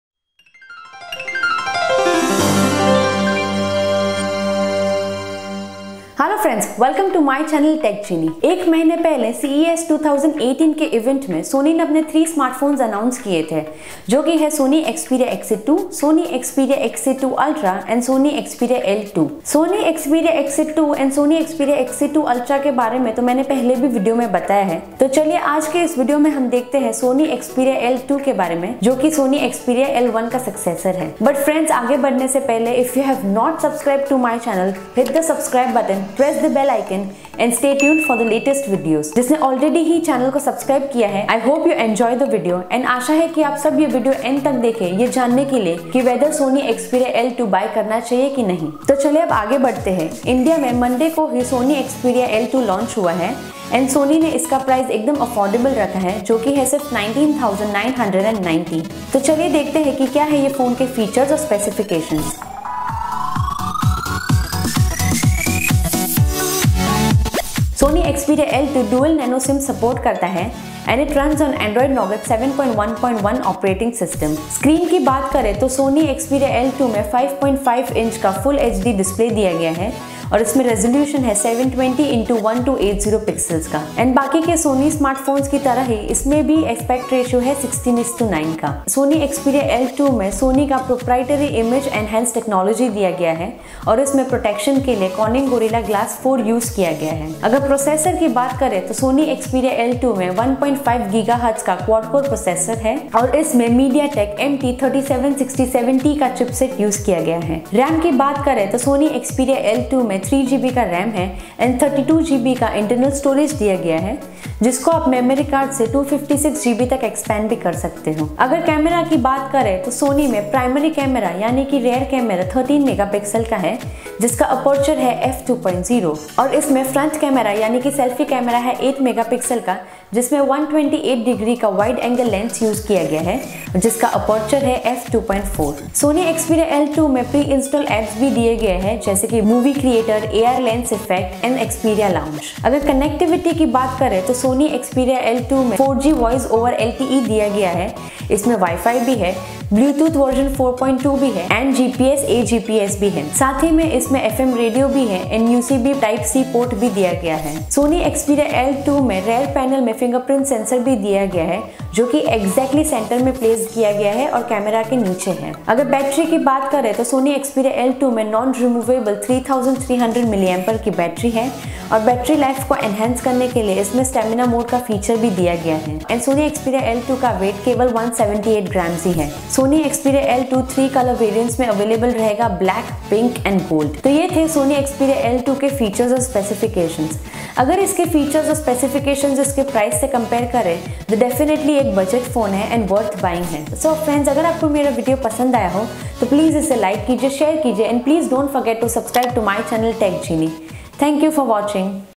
Sony Xperia L2 Official Specifications, Price & Launch Date Hindi | Sony Xperia L2 Detailed Features
New Sony Xperia L2 Latest Update, concept trailer introduction with Phone Specifications and Characteristics, the flagship of the Japanese company Sony. The Sony Xperia L2 is basically Best Mid Range Smartphone 2018. The new Sony Xperia L2 will have a 5.5-inch TouchScreen display with an 16:9 aspect ratio, a 13-megapixel rear camera, 8MP front camera and many other great features. Released Sony Xperia L2